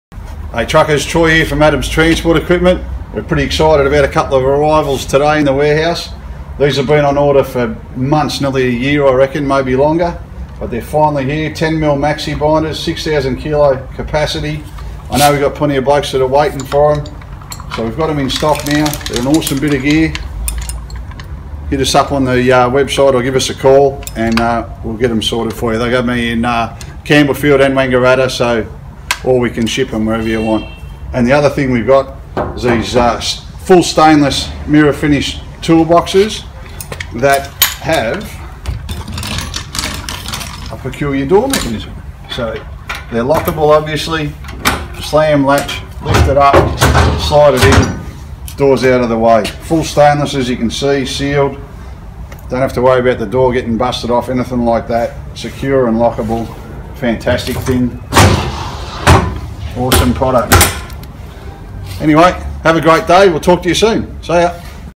Here at last - The 10mm Maxibinder With 6,000KG Capacity!
We're pretty excited about a couple of arrivals today in the warehouse.  These have been on order for months nearly a year I reckon but they are finally here. The 10MM Maxibinders with 6,000kg capacity.

Apart from Maxibinders, we got full stainless mirror-finished toolboxes that have a lockable door.  Just slam the latch and slide it and the door is out of the way.  Sealed and no need to worry about the door getting busted off.

These products are now available at Adams Transport Equipment.

For orders, give us a call.

Have a great day and we'll talk to you soon.